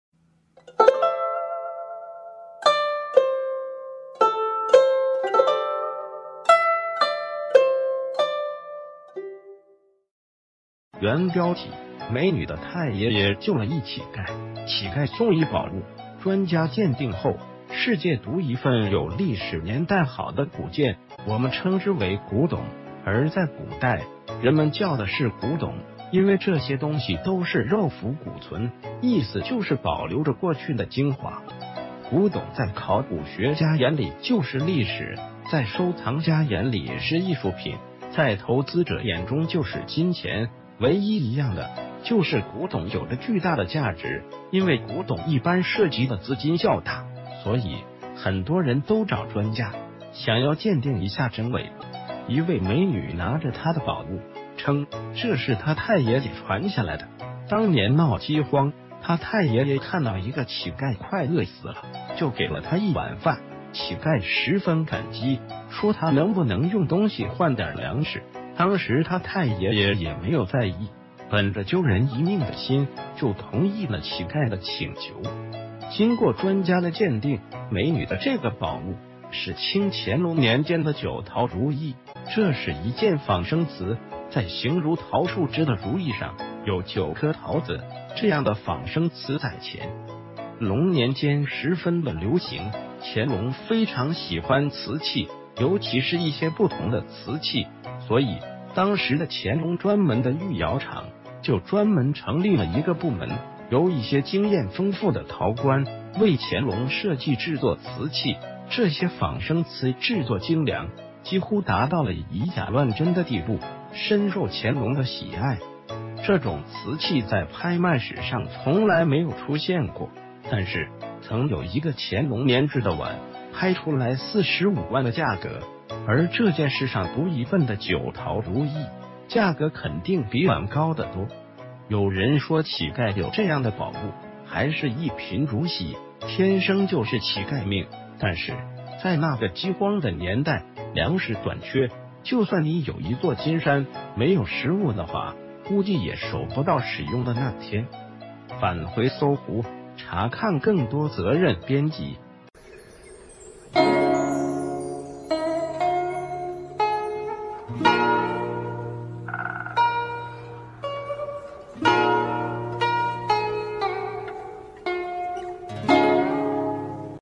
美女的太爺爺救了一乞丐，乞丐送一寶物，專家鑒定后：世界獨一份_古董
原標題：美女的太爺爺...